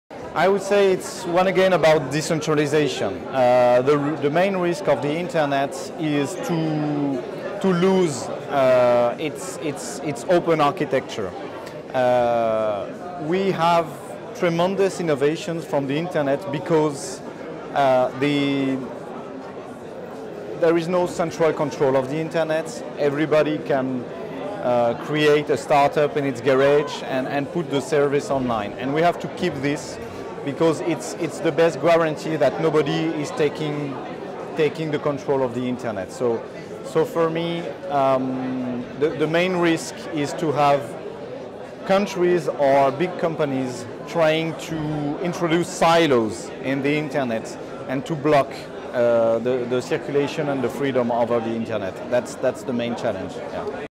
Global IGF 2018: Sébastien Soriano on Key Internet Challenge Today
Sébastien Soriano, chairman of ARCEP, France, shares their answer to the question, “What is the one most important Internet Challenge or Issue Globally today and why? What actions can be taken to address it?” Recorded at the Global Internet Governance Forum 2018 in Paris, France, by the Imagining the Internet Center, an initiative of Elon University, North Carolina, USA.